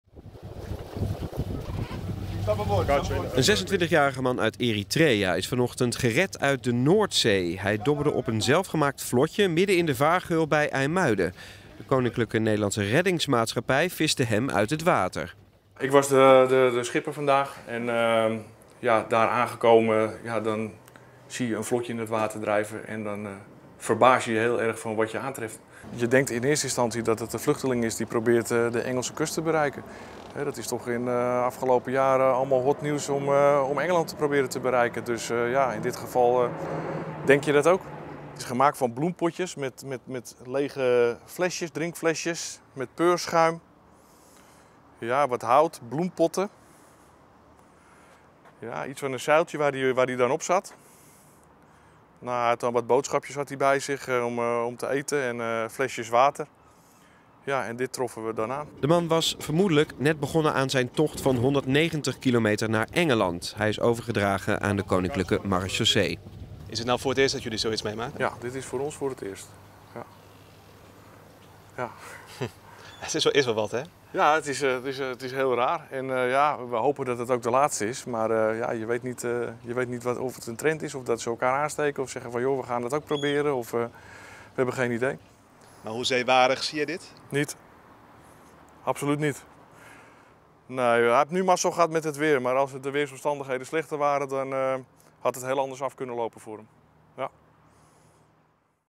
Vluchteling probeert op zelfgemaakt vlot Noordzee over te steken
Een man op een zelfgemaakt vlot is vanochtend uit de Noordzee gered door de Koninklijke Nederlandse Redding Maatschappij (KNRM). Het gaat om een 26-jarige man uit Eritrea die vanuit IJmuiden naar Engeland wilde varen.

Heb je een tip of een bijzondere foto? WhatsApp ons op: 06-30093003!